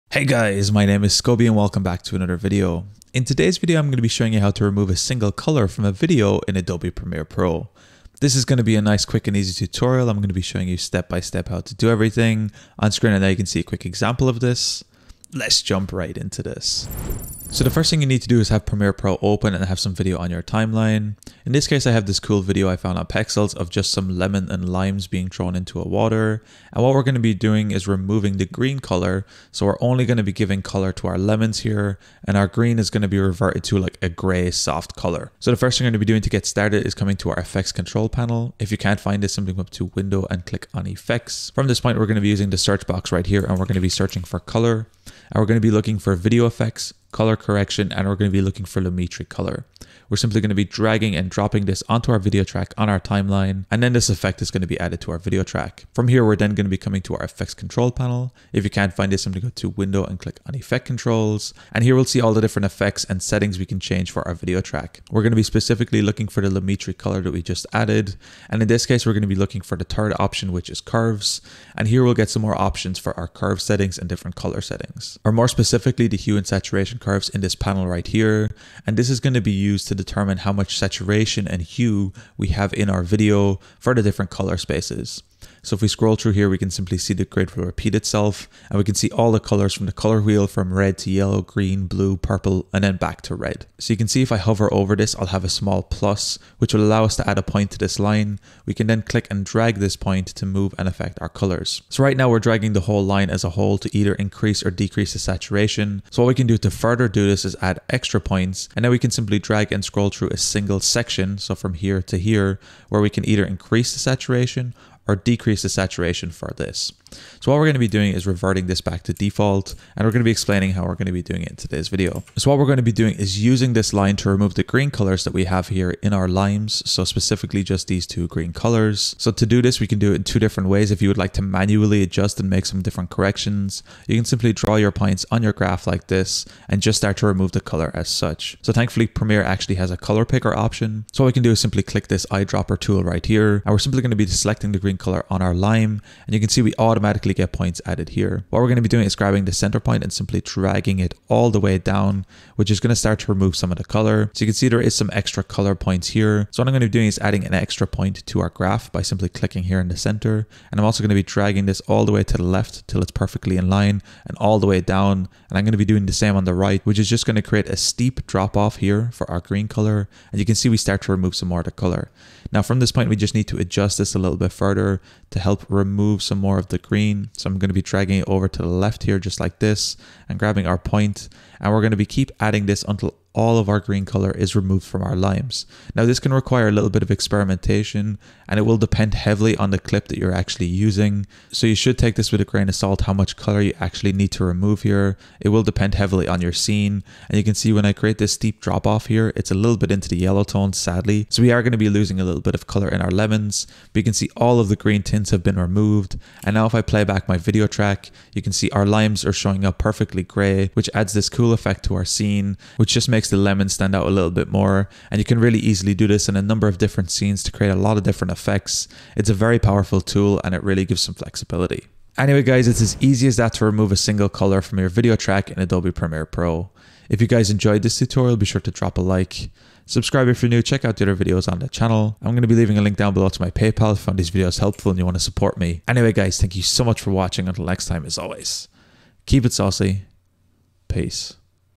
How to Remove a Single Colour From a Video in Premiere Pro(NEW LAYOUT)! Colour Removal Premiere Pro!
Hey, guys, my name is Scoby Tech and in today's video, I am going to be showing you how to remove a single colour from a video in Adobe Premiere Pro!